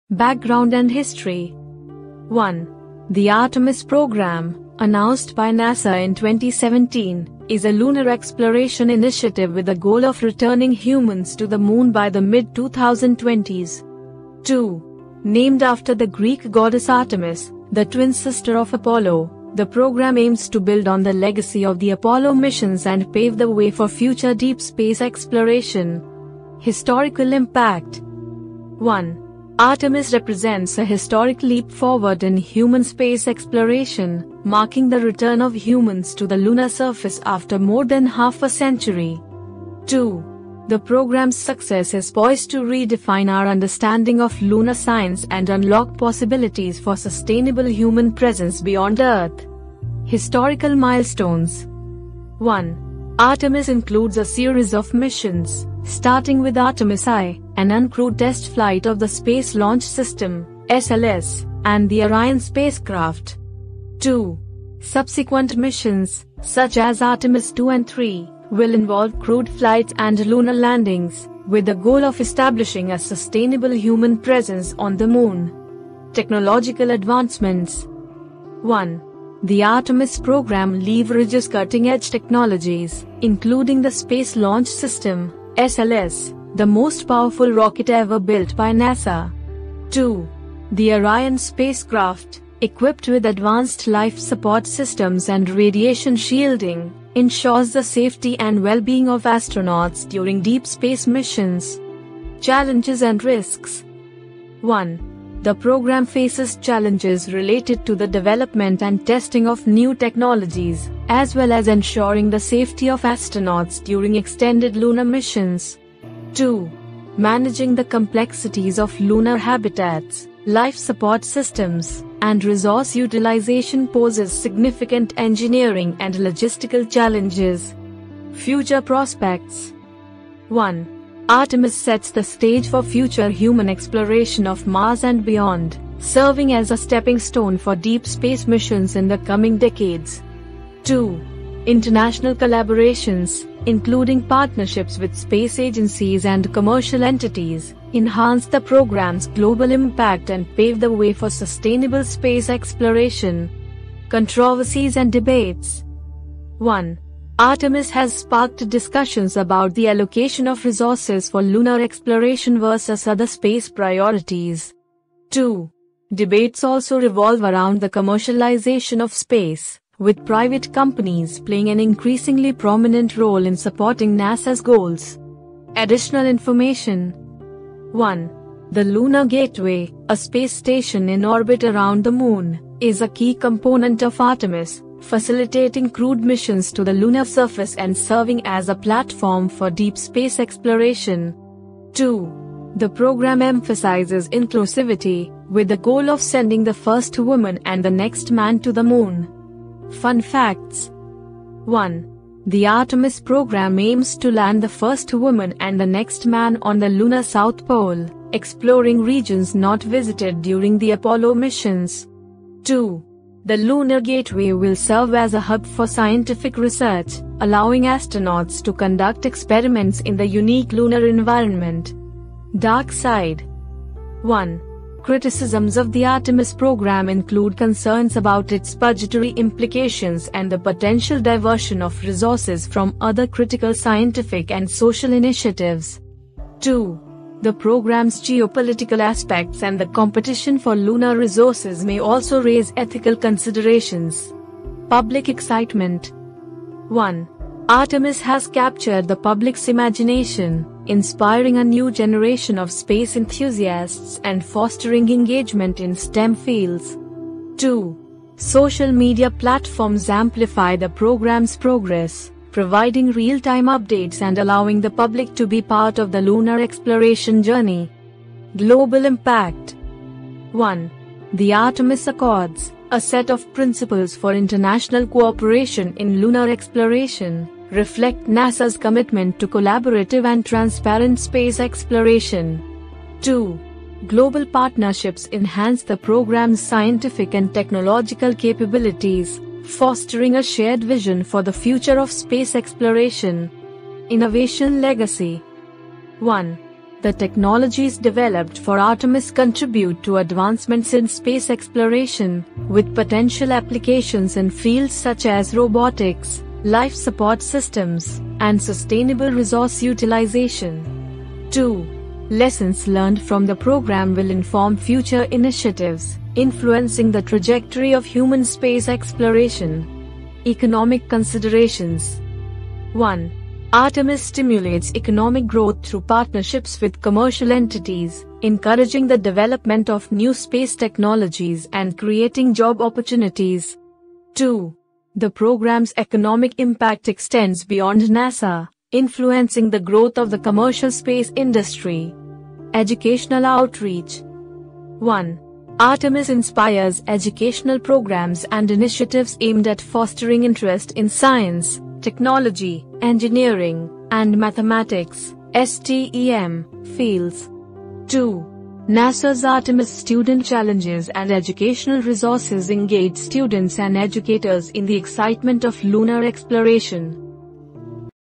Artemis Program: NASA's Odyssey Back to the Moon
Embark on an exhilarating space journey with NASA's Artemis Program, a visionary initiative to return humans to the Moon. Uncover the historical significance, cutting-edge technologies, and the promising future of human space exploration.